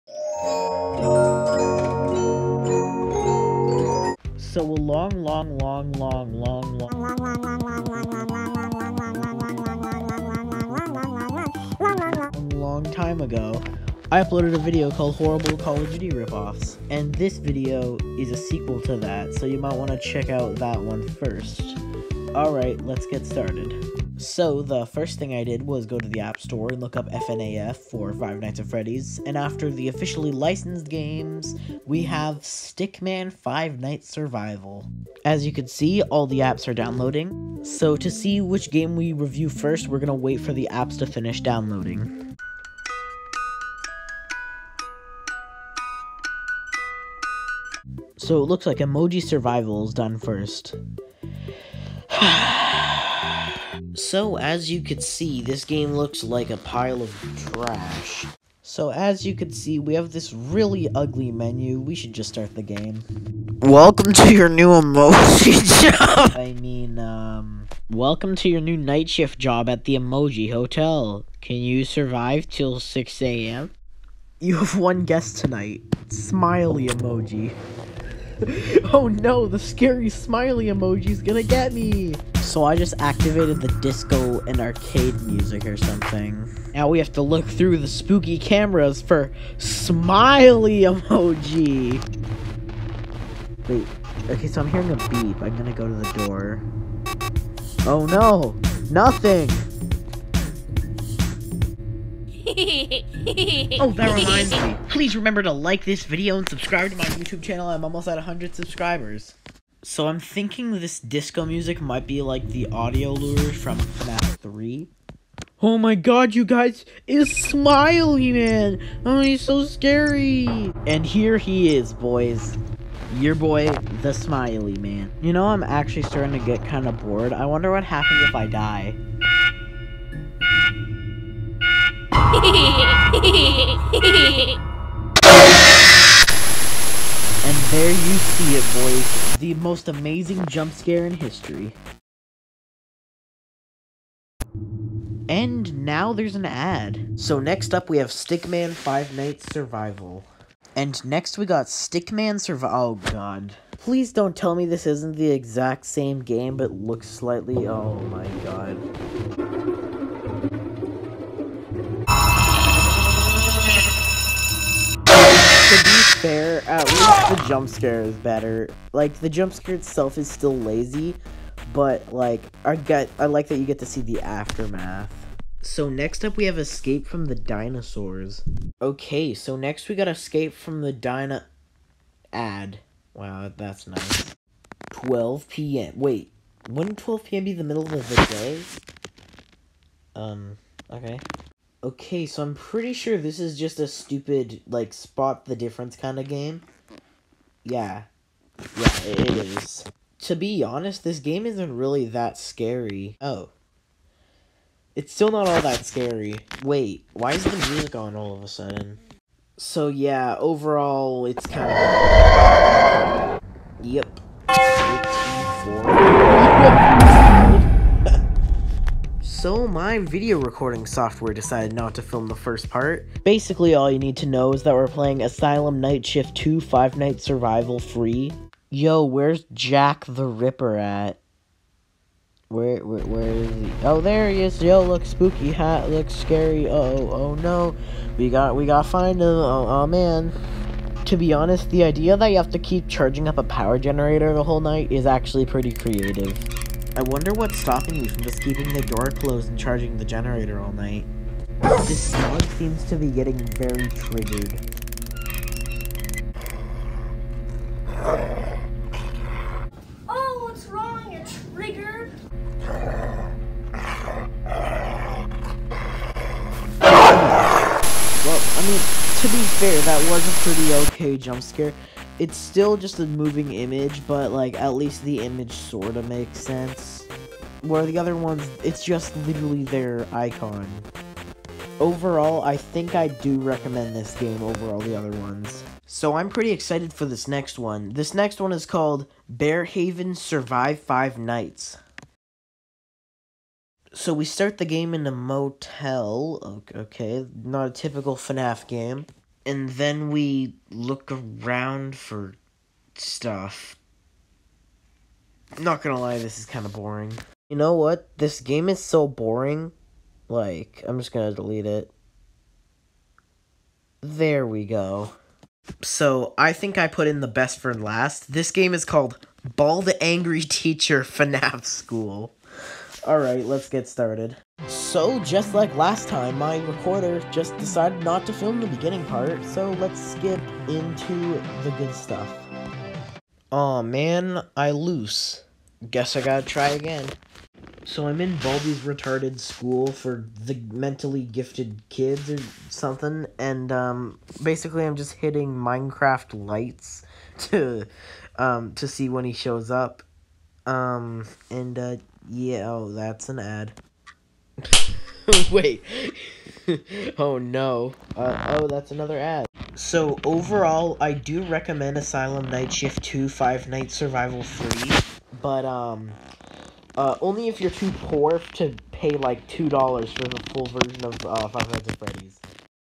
TERRIBLE FNAF RIP-OFFS
In this video I take a look at Rip-offs of the iconic horror series Five Nights at Freddy's.